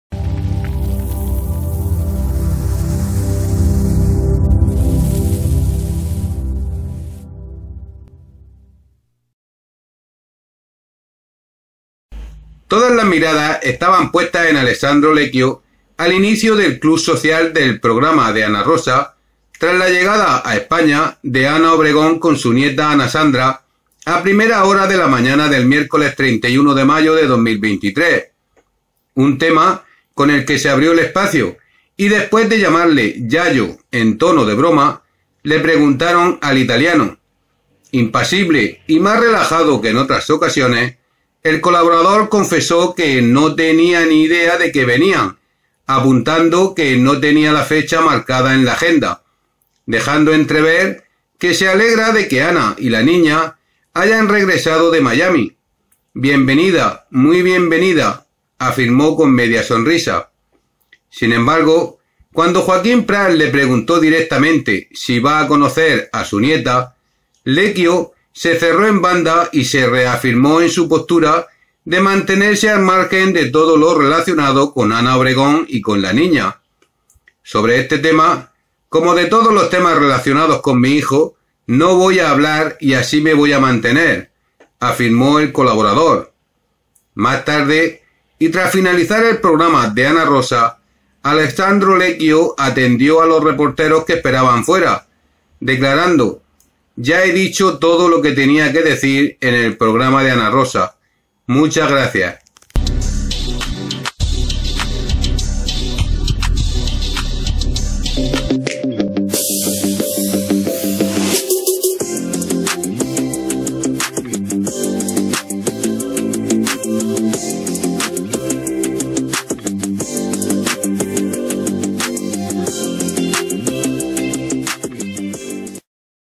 Alessandro Lequio reacciona con frialdad al regreso de Ana Obregón
Alessandro Lequio ha decido quedarse al margen de todo lo relacionado con Ana Obregón y la niña Ana Sandra.

01/06/2023